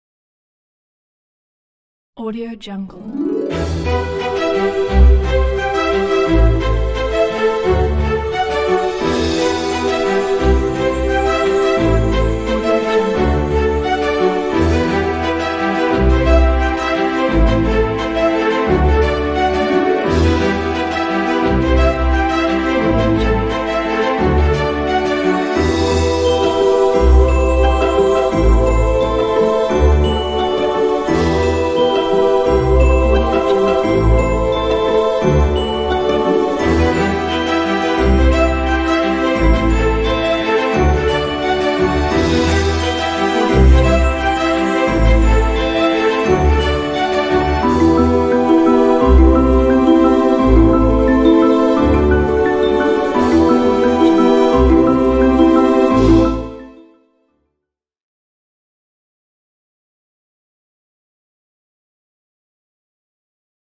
VideoHive - Happy Chocolate Day - 32280157
«Happy Chocolate Day» - шаблон для создания презентации дня шоколада.